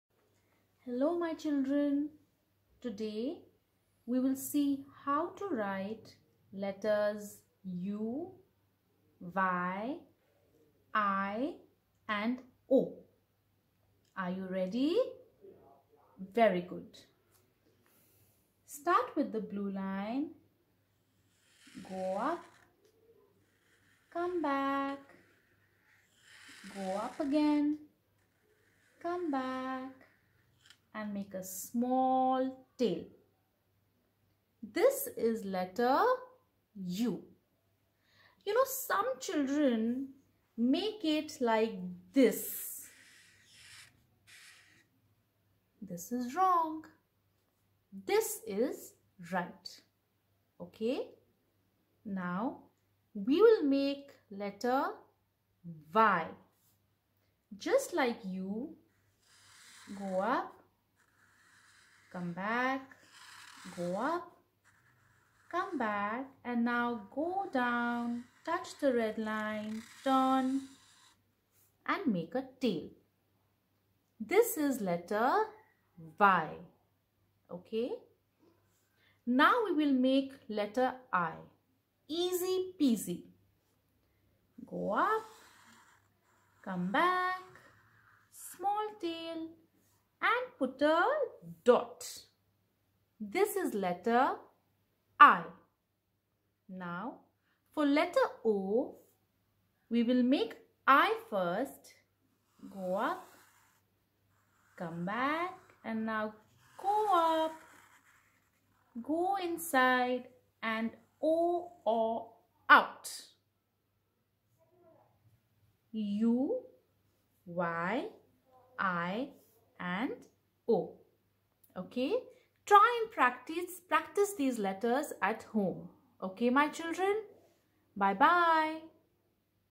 Writing letters u y i o
Correct formation of letters u y i o in small cursive.